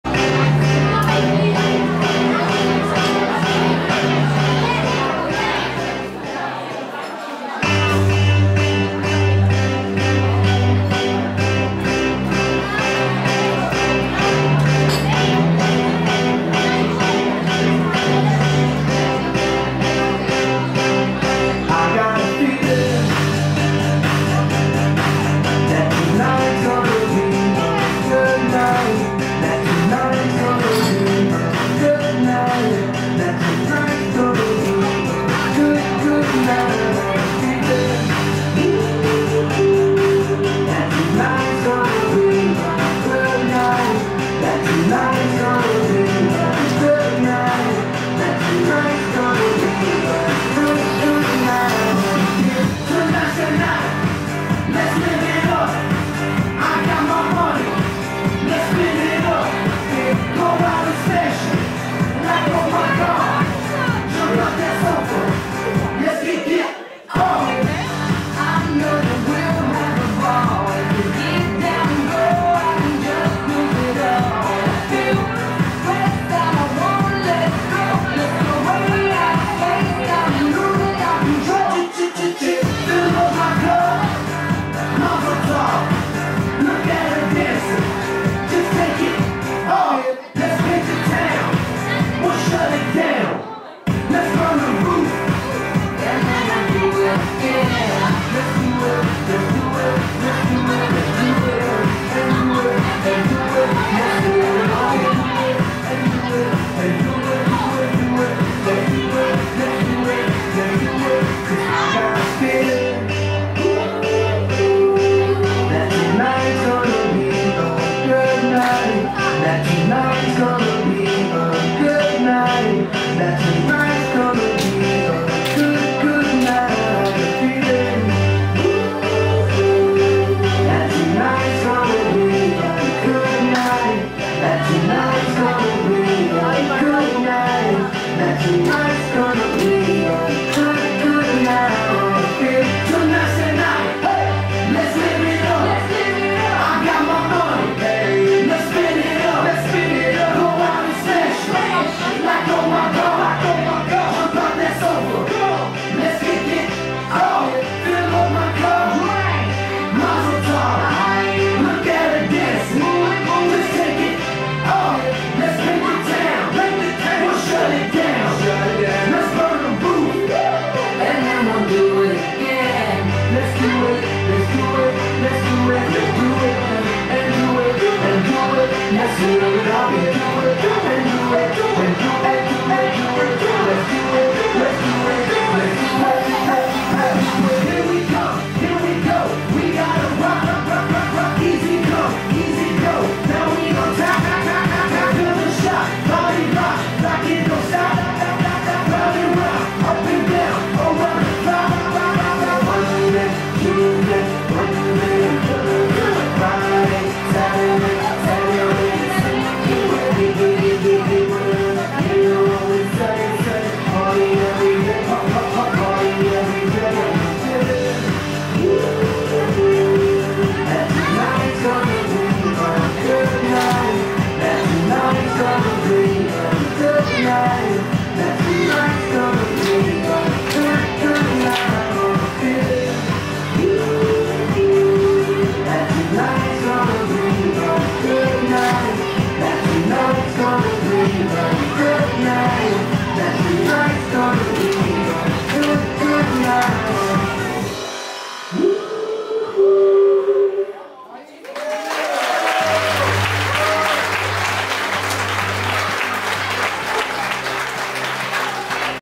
MooiMaasdriel.nl / Openingsshow door groep 8
Openingsshow door groep 8 op De afsluiting van het schooljaar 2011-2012